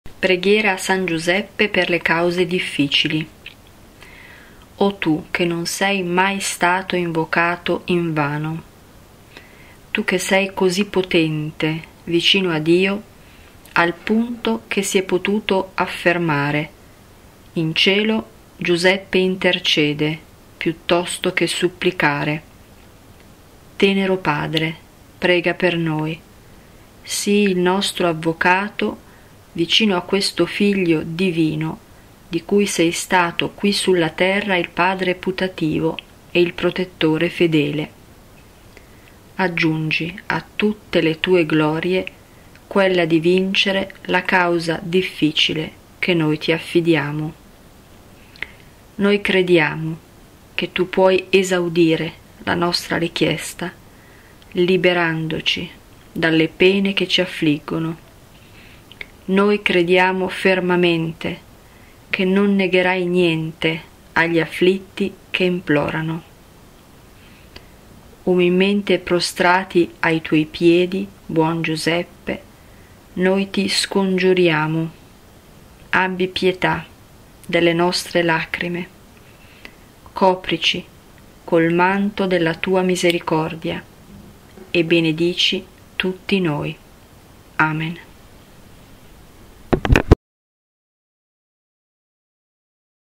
PREGHIERA A SAN GIUSEPPE per le cause difficili
O Tu, che non sei mai stato invocato invano!

Tu che sei così potente vicino a Dio al punto che si è potuto affermare: "In Cielo, Giuseppe intercede piuttosto che supplicare", tenero padre, prega per noi; sii il nostro Avvocato vicino a questo Figlio divino di cui sei stato qui sulla terra il padre putativo ed il protettore fedele; aggiungi a tutte le tue glorie, quella di vincere la causa difficile che noi ti affidiamo!

Noi crediamo che Tu puoi esaudire la nostra richiesta, liberandoci dalle pene che ci affliggono.

Noi crediamo fermamente che non negherai niente agli afflitti che implorano.

Umilmente prostrati ai tuoi piedi, buon Giuseppe, noi ti scongiuriamo: abbi pietà delle nostre lacrime; coprici con il manto della tua misericordia e benedici tutti noi. Amen.

"GIUSEPPE DI NAZARETH. LA VITA, LE VIRTÙ, LA GLORIA”.
Il link delle prime trasmissioni andate in onda su